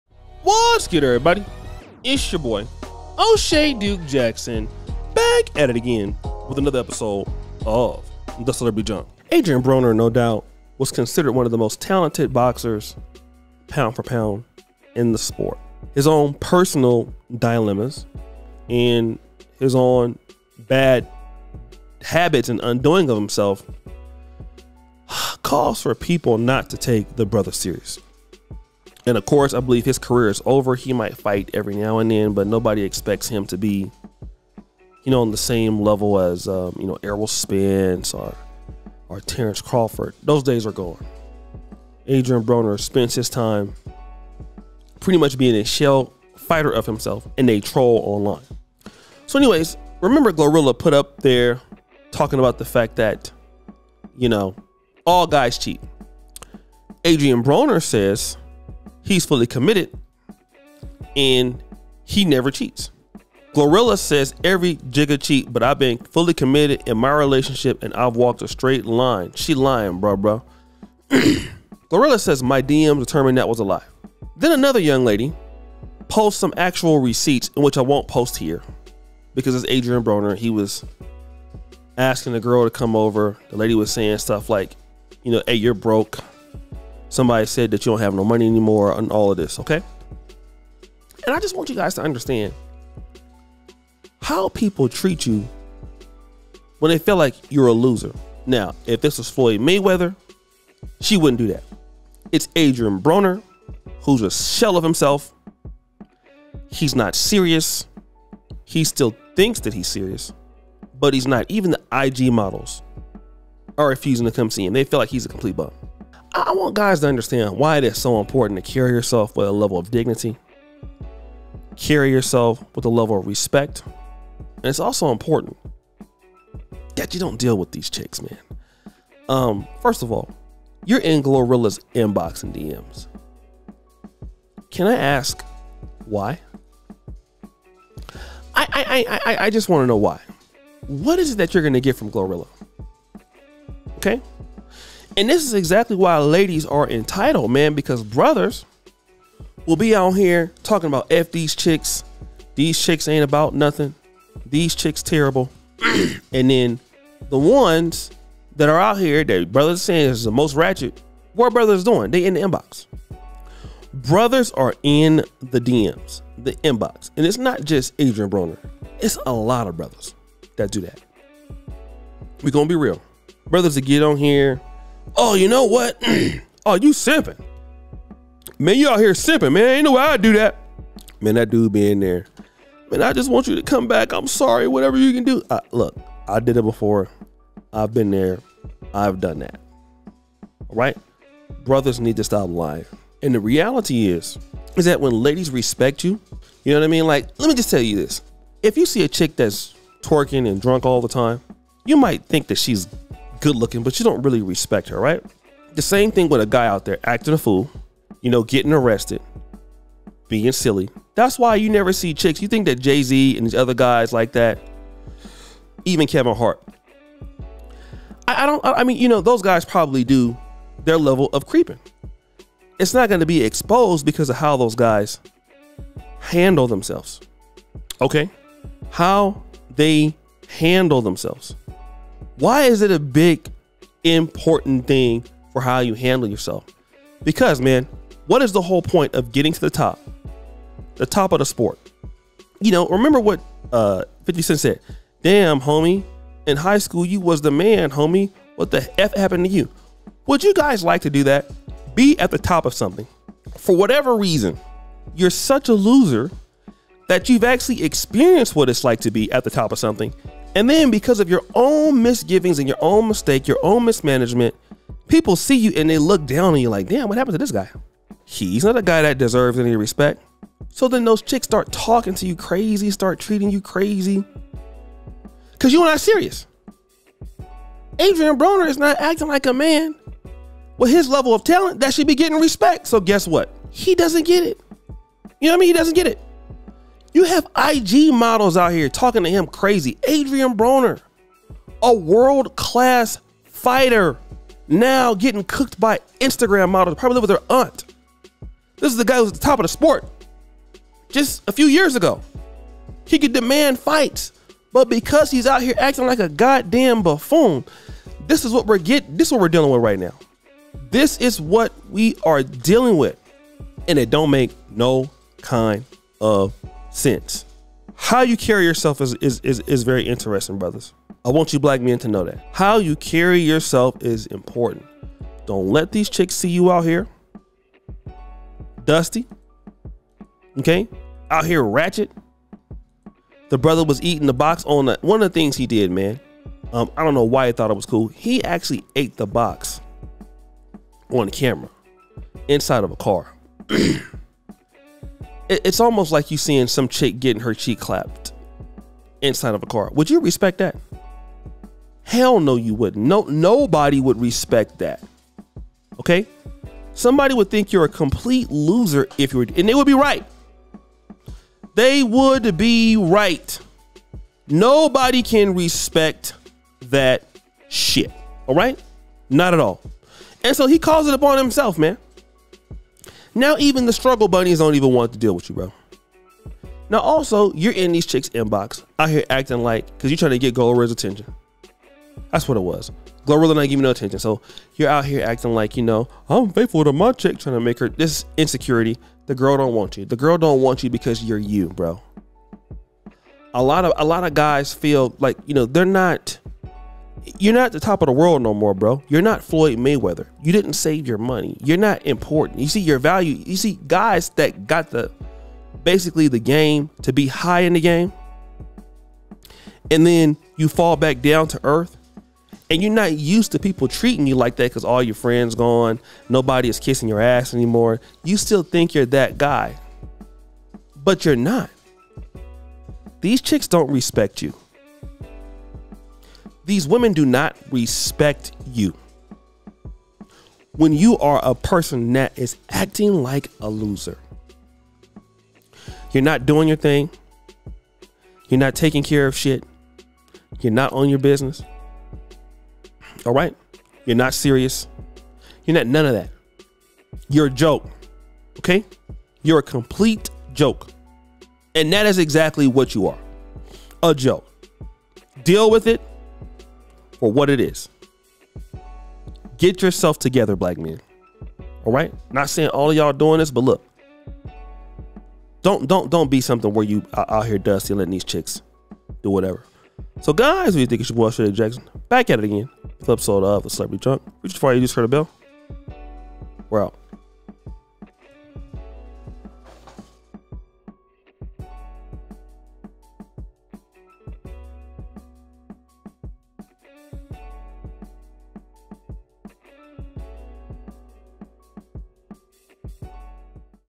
Glorilla Exposes This Famous Guy For Trying To Clap Her Cheeks in DMS
GloRilla has seemingly exposed Adrien Broner after the boxer disputed the rapper’s claims about the alleged infidelity epidemic amongst men.

On Tuesday (January 23), the boxer took to his Instagram stories to illustrate that he was a faithful boyfriend. “@GloRillaPimp say every n-gga cheat, but I’ve been fully committed in my relationship, I’ve walked a straight line, she lying bra bra.”

Broner was referring to the Memphis rapper’s tweets on Monday (January 22), in which she claimed, “every n-gga cheat. It’s not a faithful nigga walking dis earth stamp dat. I got cheated on 1 time in my whole life, so no dis ain’t got shit to do with me I’m not da problem [laughing face emoji].”

Glo, however, found out about Broner’s comments on Instagram, and immediately took to her own stories to call shenanigans on his faithfulness claims. “My DMs determined dat was a lie,” she wrote on a screenshot of his story, which she subsequently shared with her followers.